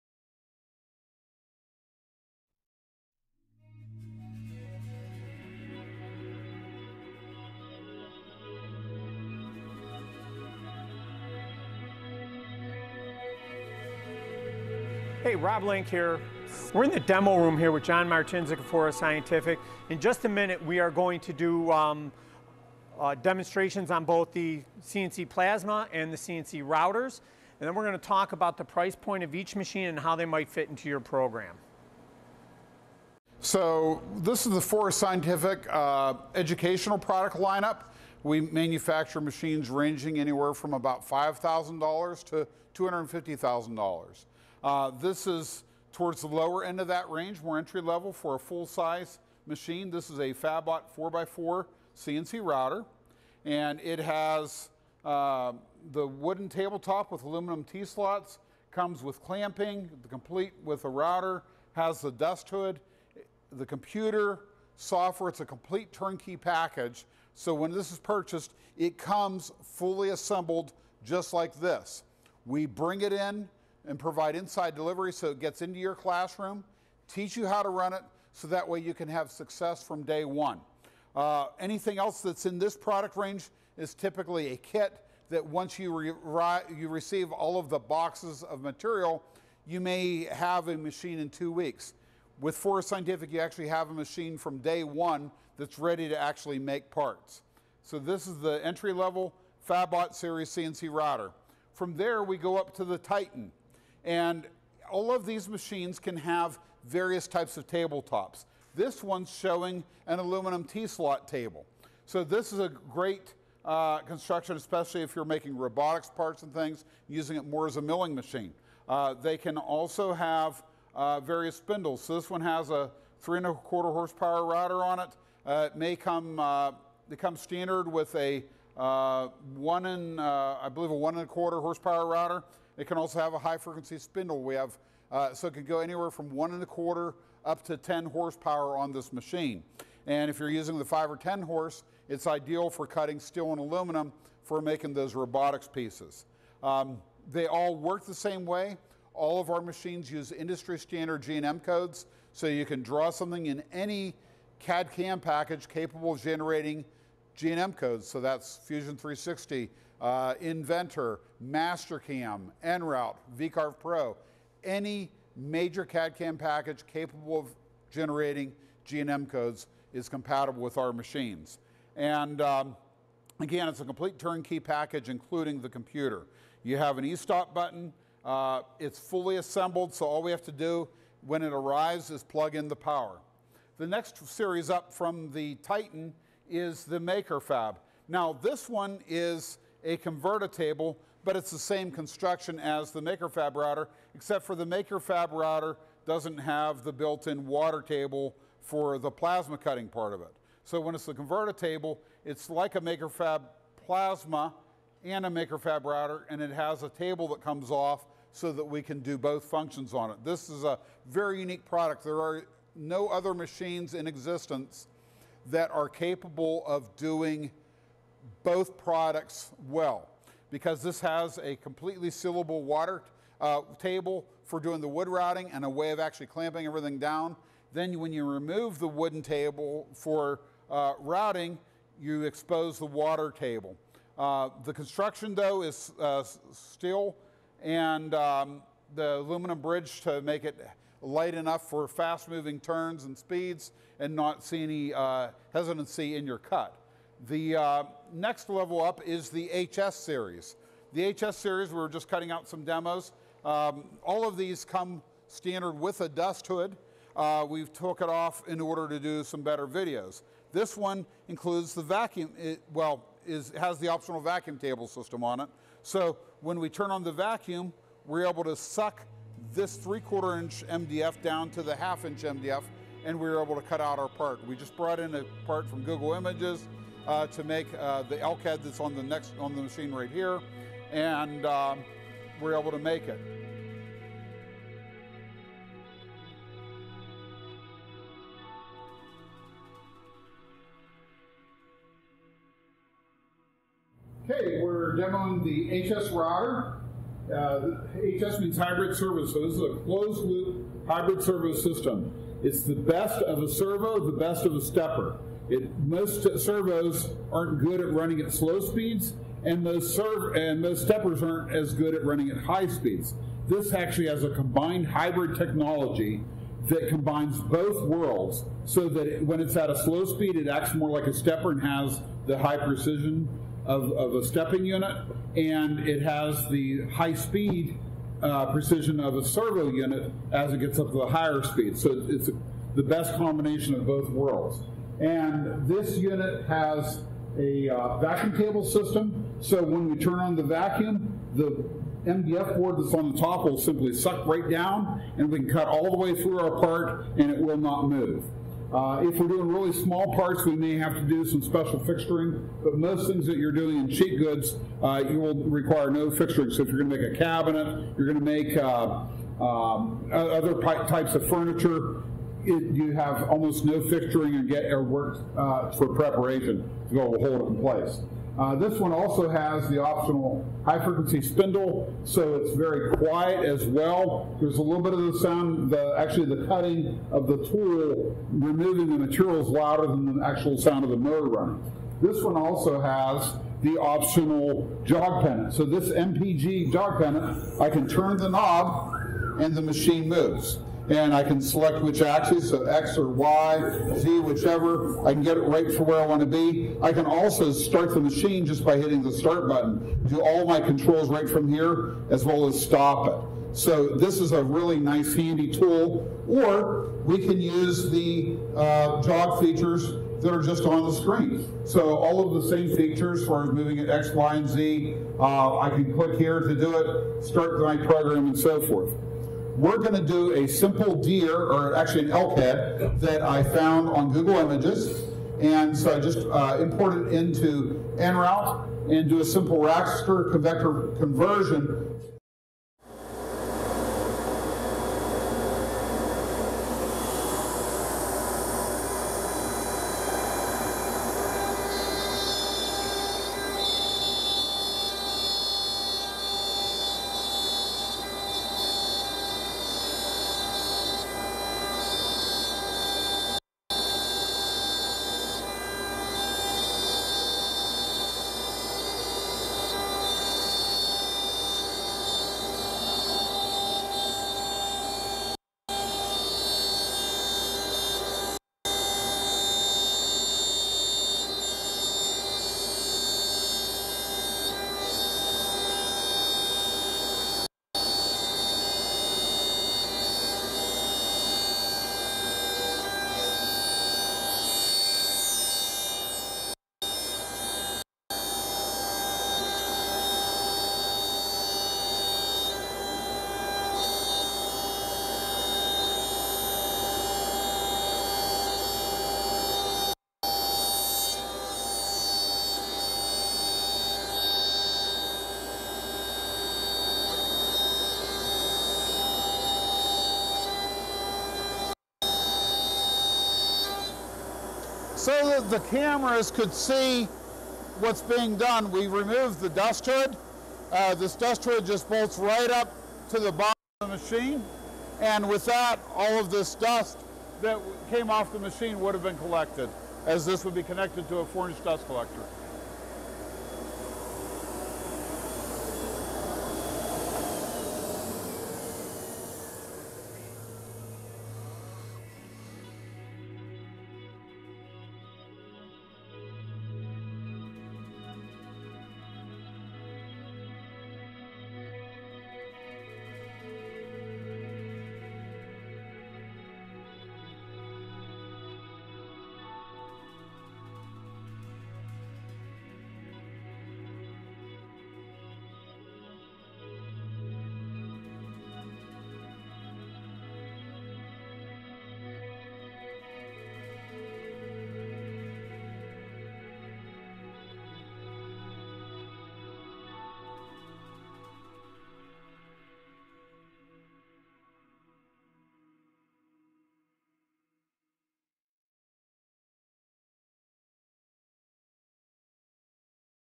Forest CNC Router Overview and Demonstration
Rob Link from Allegheny Educational Systems is with John Martincic of Forest Scientific to preview the full line of CNC Routers and do a demonstration of the Forest HS Series CNC Router.